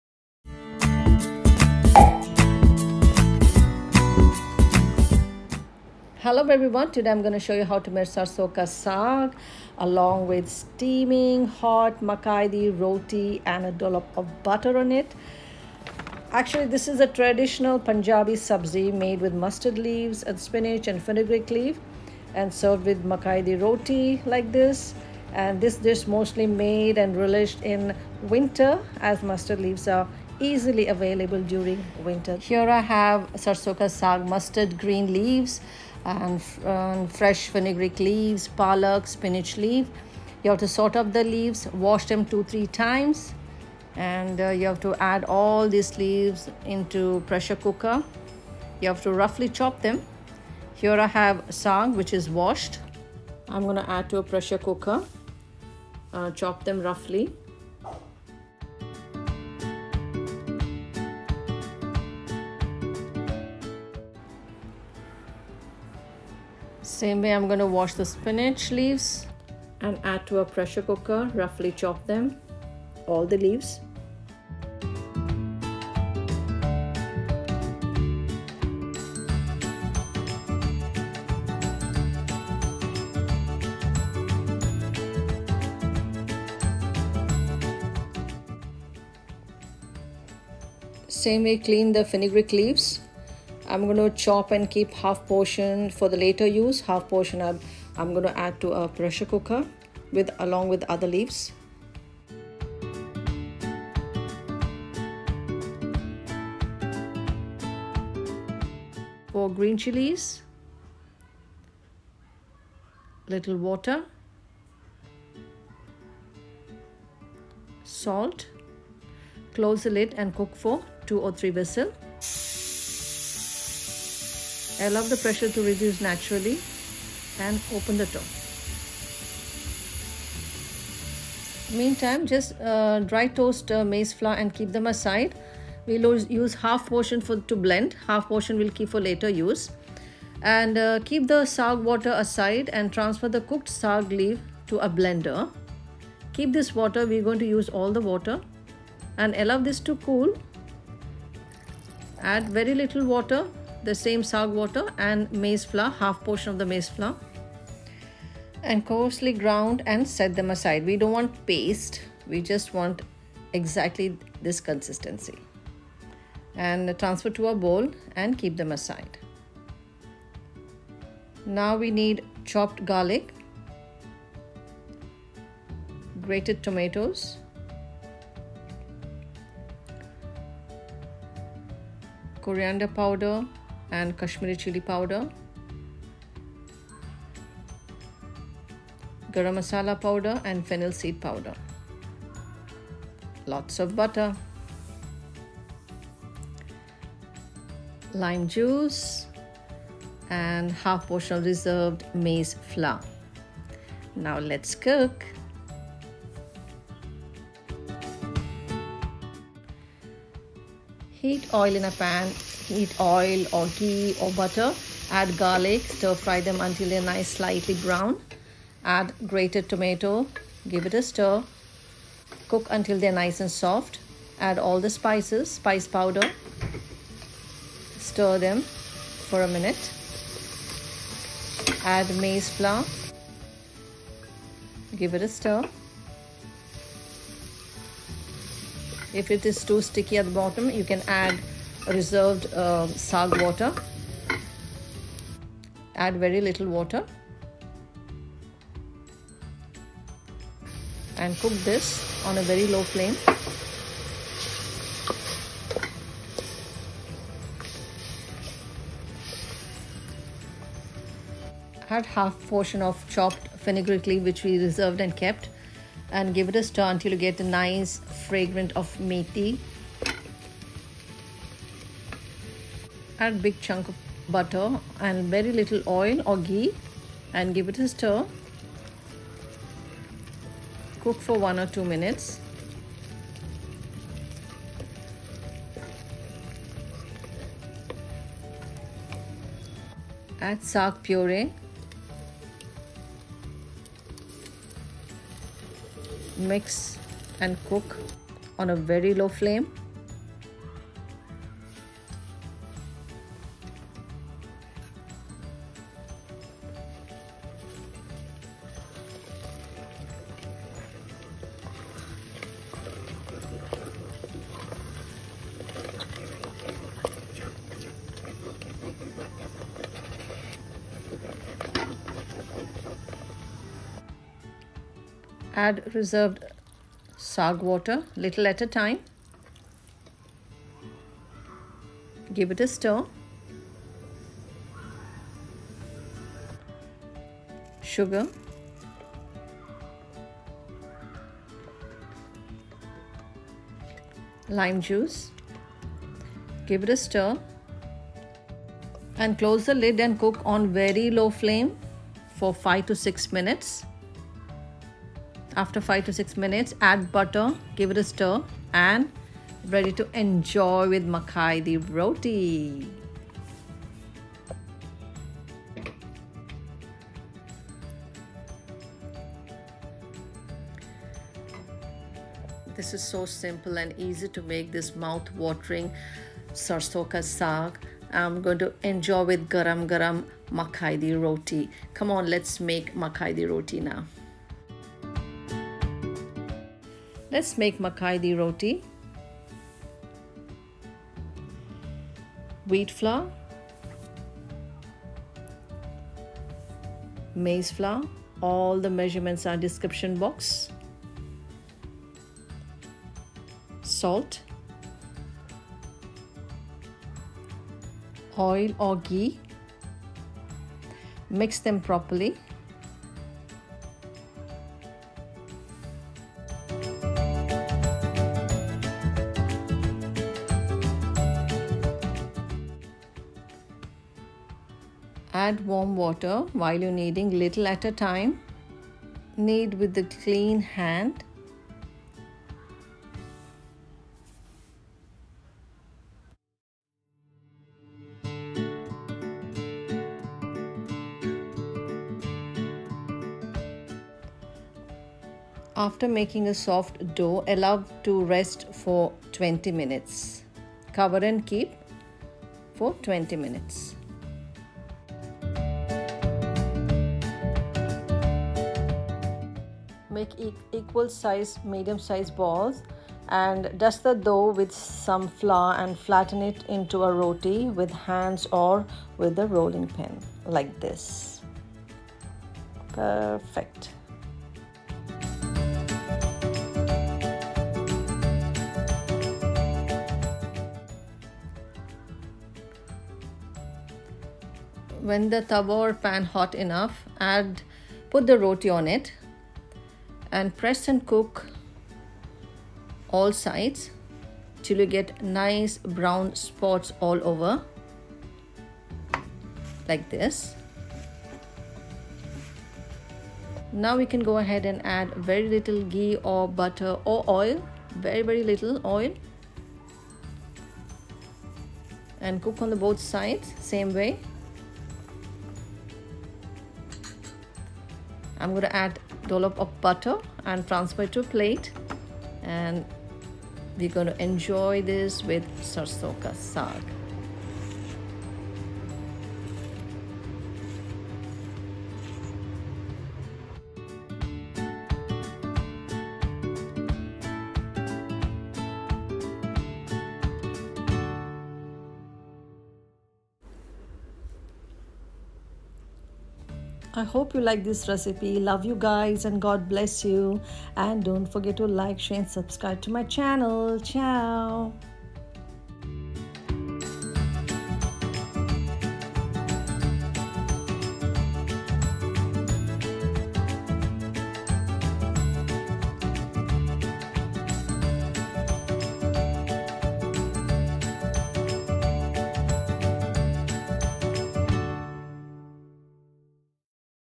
Sarson ka Saag with Makki Di Roti, Spinach Saag, Palak Saag
Sarson ka saag with Makkai di Roti
With a dollop of butter.

Ingredients
(step 1)
-1 bunch Sarson saag/mustard green leaves
-1 bunch fresh fenugreek Leaves
-2 bunches palak/spinach leaves
-4 green chilies

-Sort out the green Wash & clean all the leaves 3-4 times l, cut and add into pressure cooker sarson saag, Palak, fenugreek leaves half portion (half for later use), green chilies, salt, 1/2 cup water
-Cook over medium heat to 2
Whistles, turn off the flame, allow pressure come down naturally
-Open the lid keep saag water aside add transfer cooked saag leaves to a blender, with toasted 3 tbsp makkai atta, very little saag water Coarsely ground and set aside

-SPICE POWDER OTHER INGREDIENTS
-10 garlic cloves chopped
-1/2 portion reserved fenugreek leaves chopped
-2 medium size tomatoes grated
-1/2tsp cumin power
-1 trap coriander powder
-1/2 tsp homemade Garamond masala power
-1/2 tsp homemade fennel powder
-dash turmeric powder
-1 tsp Kashmiri chili powder
-1/2 lime juice
-3 tbsp toasted Makkai
Atta/Maize flour
-2 tbsp butter
-4 tbsp oil

-Heat oil in a pan , add garlic stir till slightly brown in color, add tomato stir until tomato are soft, add spice powders stir, add toasted Makkai atta stir for 1 second, add chopped fenugreek leaves stir and cook until fragrant, add coarsely grounded saag stir and cook for 2 minutes  add butter, salt, lime juice and saag water close the lid and cook over very low flame for 5 more minute
-Adjust taste..
-Serve with hot Makki Roti & dollop of butter on top of the Sarson Di Saag.

Let’s make 🫓
MAKKI ROTI
1 cup wheat flour
1 cup makki ki  atta /Corn meal/maize flour
-warm water for kneading

-In a wide kneading bowl add maize flour, wheat flour, salt  add warm water very little at a time, knead with clean hand after making a soft dough allow to rest for 20 minutes
-Then make a equal portions of dough and make medium size balls
Dust the dough with some flour & flatten it into a roti with hands or with a rolling pin
-Heat tawa/pan over medium flame & let it heat
when the pan is hot enough put the roti on it & cook adding very little oil/butter
Cook on both the side till you get brown spots, when roti is cooked transfer In a serving plate apply butter on the roti and serve hot with Sarson Do Saag